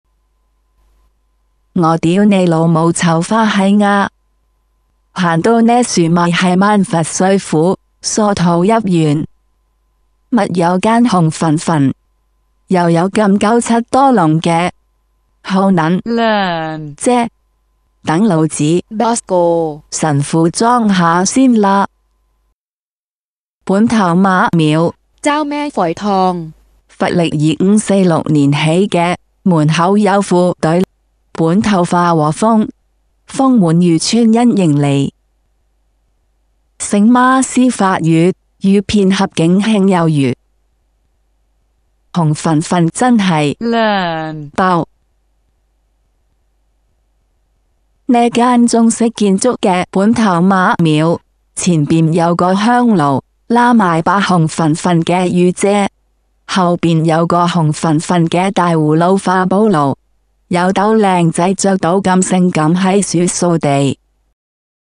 本頭媽廟（เจ้าแม่ฝอยทอง）Chao Mae Foithong Shrine, Sattahip, Chonburi
位于暹羅（Siam (Thailand)）萬佛歲府（Chonburi province）梭桃邑縣（Sattahip district）海濱之『本頭媽廟（เจ้าแม่ฝอยทอง (Chao Mae Foithong Shrine)）』，乃道教廟觀（Taoist temple），建築於佛曆二五四六年（中華民國九十二年），門口有一幅對聯：「本頭化和風，風滿漁村欣盈利；聖媽施法雨，雨遍合境慶有餘。」
地址：Soi Na Jomtien 30, Moo 3, Na Chom Thian, Sattahip, Chonburi 20250